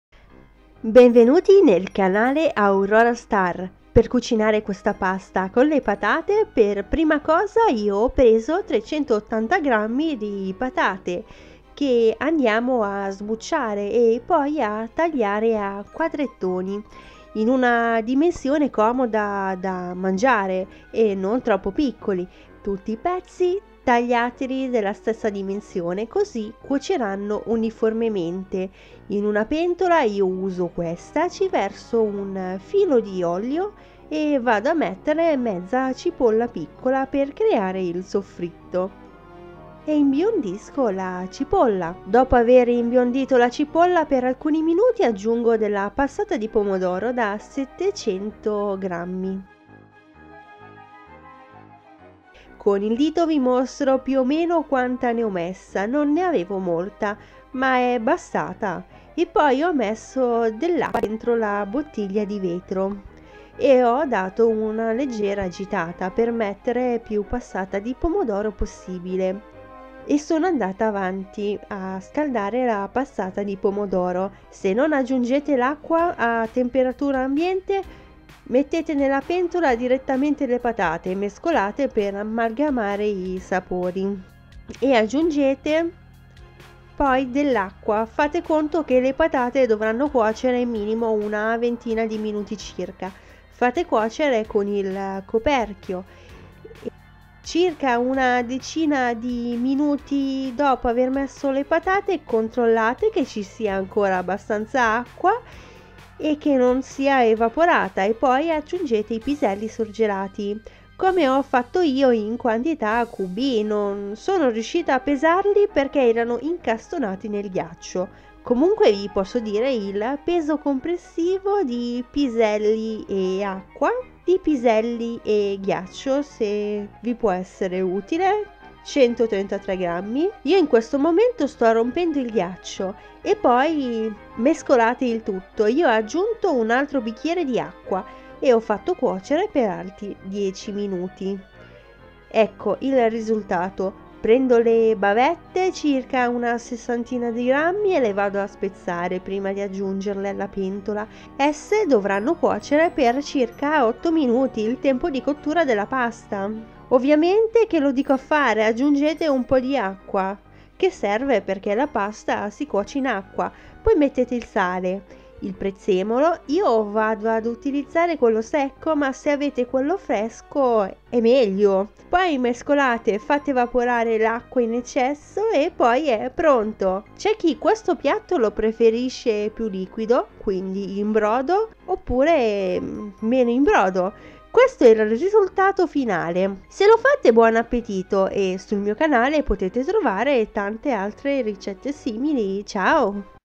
Pasta patate e piselli. Ricetta.
Un piatto invernale con con un sugo di pomodoro, patate e piselli è una pasta da leccarsi i baffi! La ricetta è semplicissima.